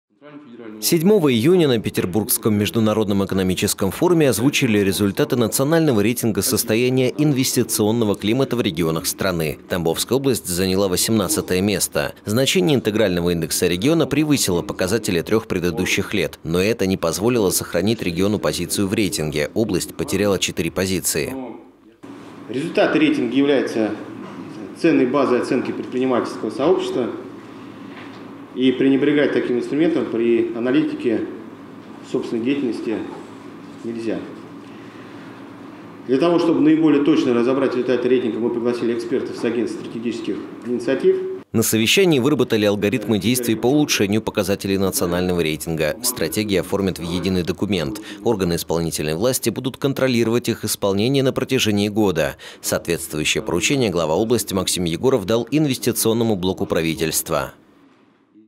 В Тамбовской области выработали алгоритмы действий по улучшению показателей Национального рейтинга
Итоги Национального рейтинга состояния инвестиционного климата в российских регионах за 2023 год обсудили на совещании в Правительстве области.